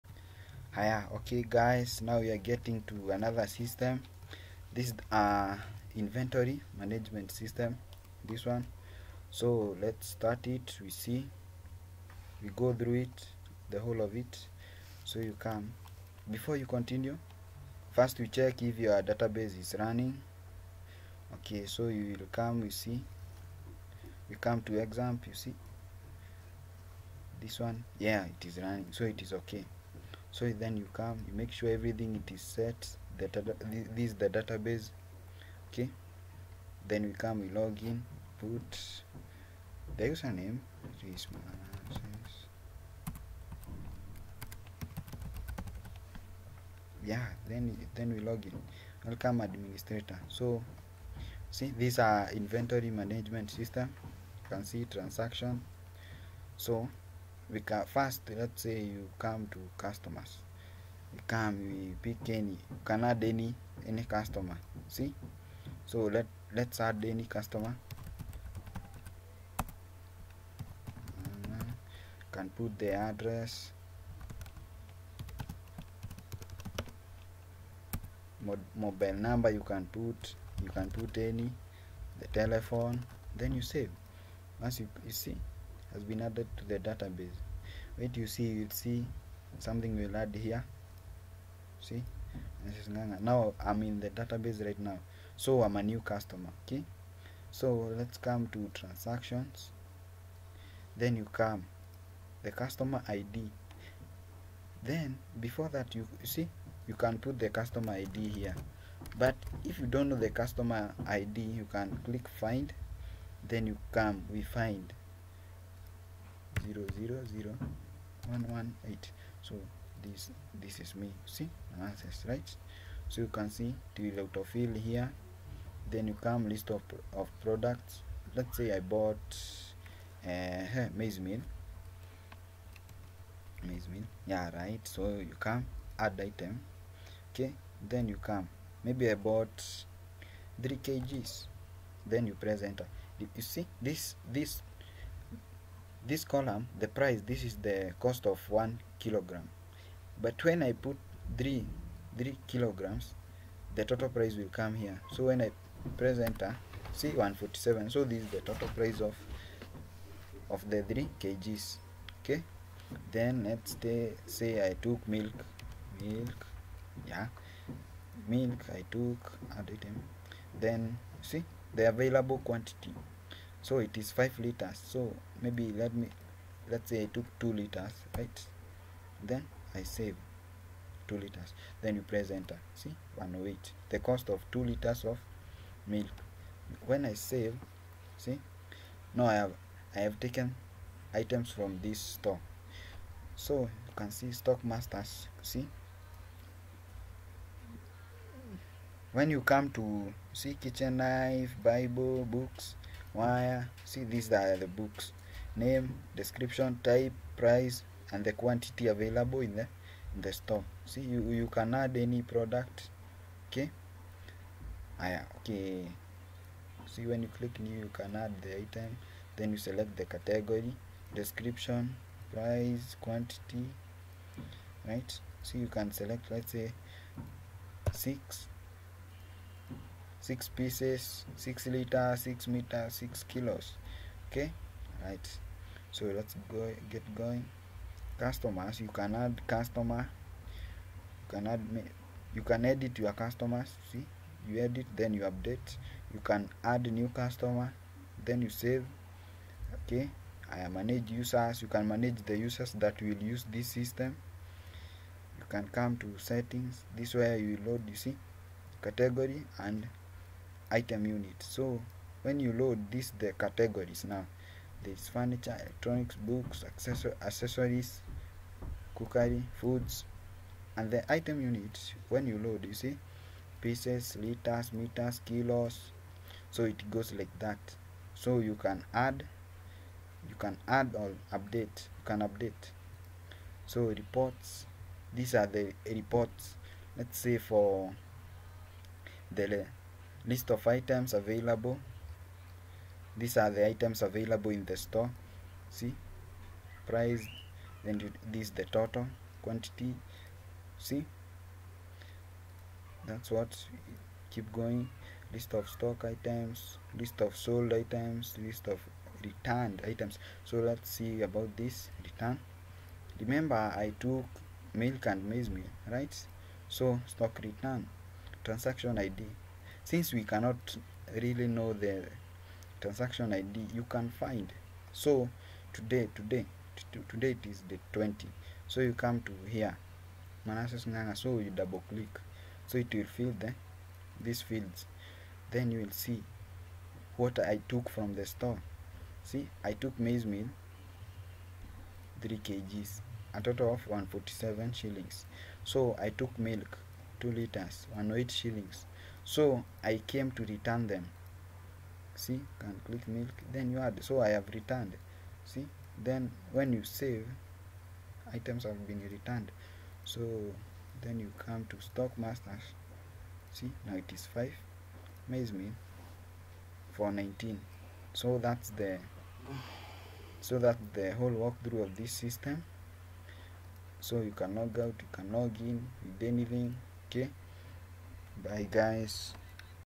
Inventory Management System
This is an inventory management system...
These are the following features of the system
Manage Transaction

Stock-out
Stock Return
Stock Master
Manage Customer
Manage Users
Settings
Reports

Analytics Reports
Inventory Reports
Sales Reports